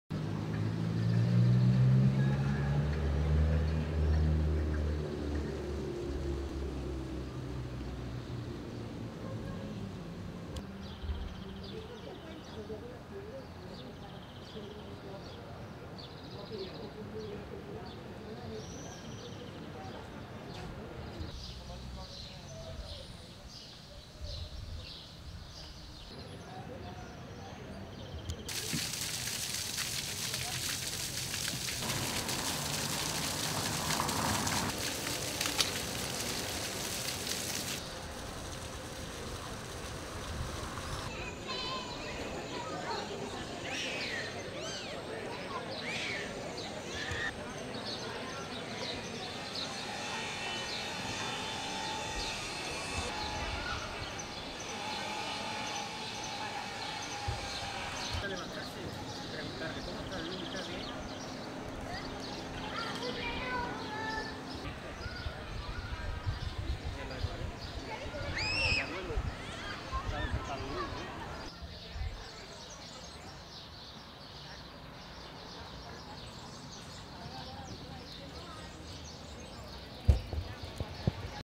Vecinos de Badajoz salen de terrazas con las altas temperaturas
La ciudad de Badajoz registra este miércoles temperaturas por encima de los 30 grados al igual que otros puntos de Extremadura; motivo por el cual algunos de sus vecinos han aprovechado para salir a tomar algo a las terrazas de los bares que las tienen abiertas al 50 por ciento como se establece para la fase 1 de la desescalada en la que se encuentra Extremadura.